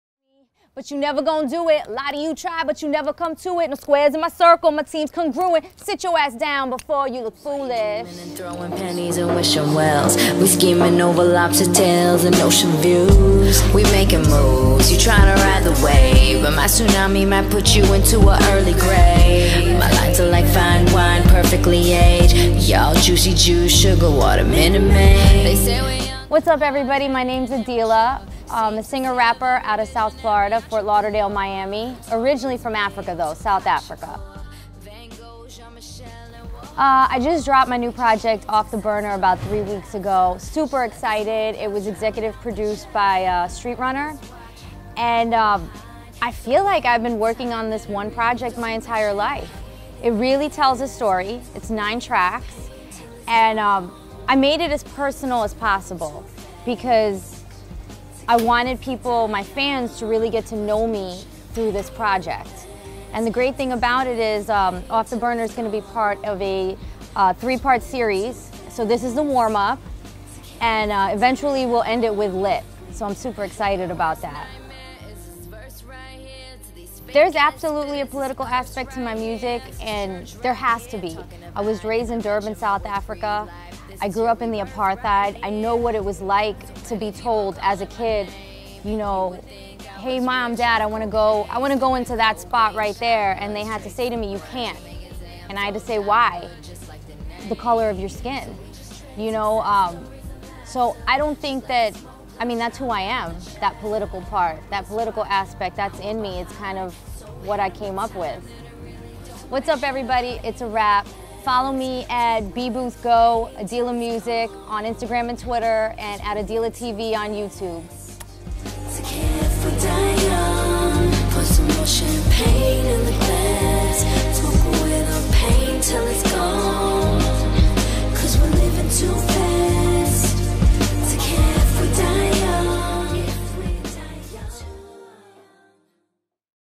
AdELA Artist Spotlight With B Booth
B Booth Go
bBooth is the intersection between entertainment, social media and technology, providing new opportunities for rising talent to engage like never before with Hollywood auditions, events and ‘insider’ content.